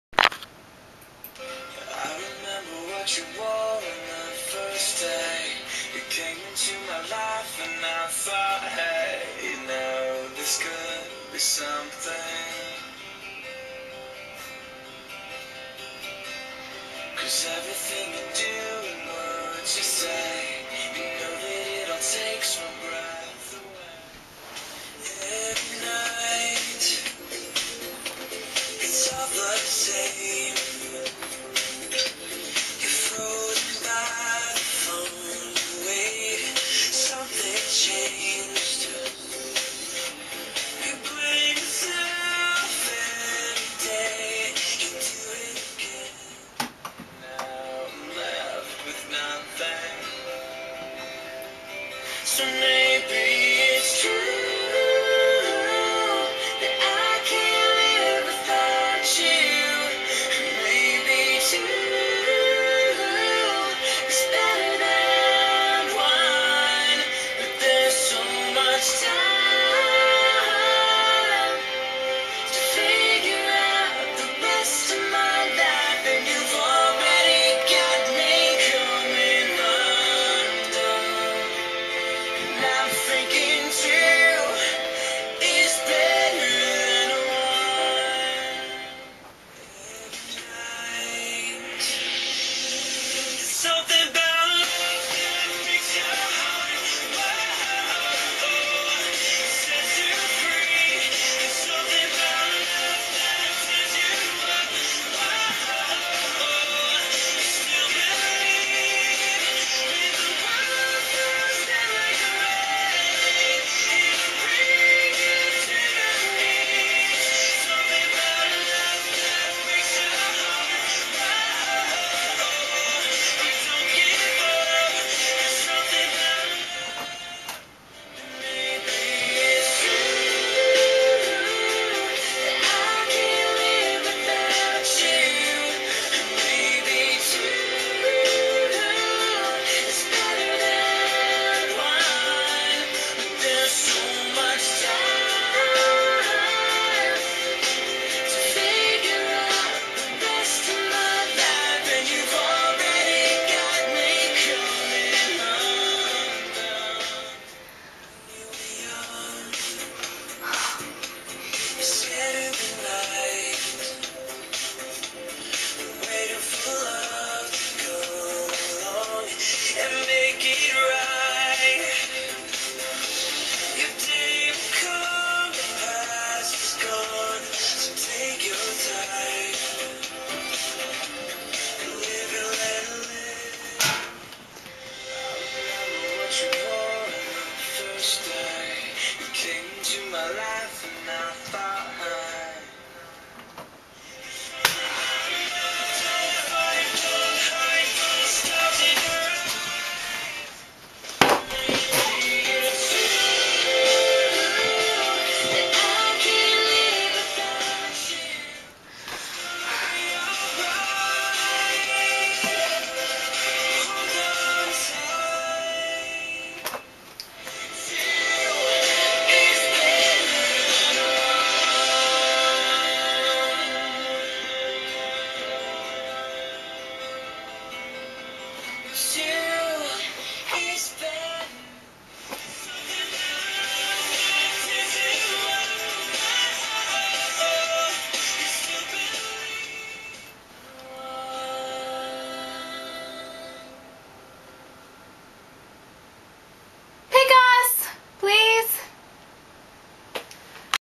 Battle of the Ticket To Ride Artists
This is our entry for the Ticket to Ride Contest for Meet and Greets. My friend likes Boys Like Girls and I like David Archuleta so we decided to have a battle of the artists.